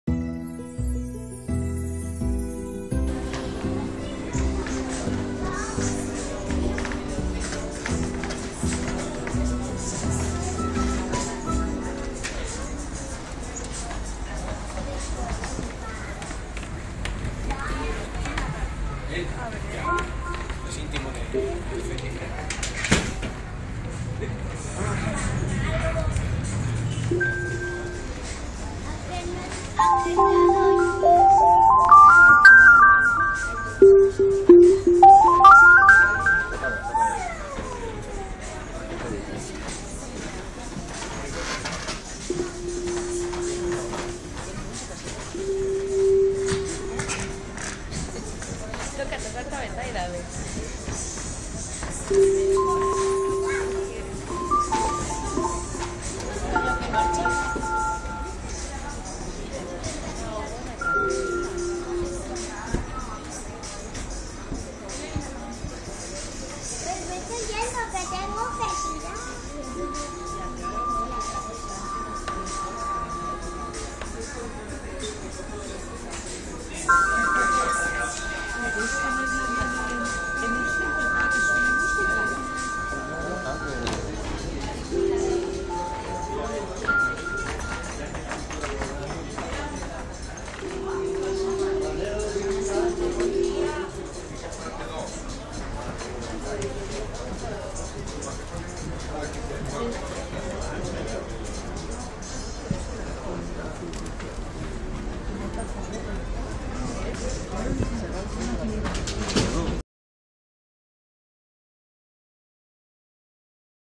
Con-Tacto en el Concurso de Pieza Única
Primeras reacciones del público ante la pieza única Con-Tacto. Están sacadas en un momentín que me escapé de mi stand, pero la oigo con frecuencia desde donde estoy. Parece que ha tenido buena acogida, mucha participación.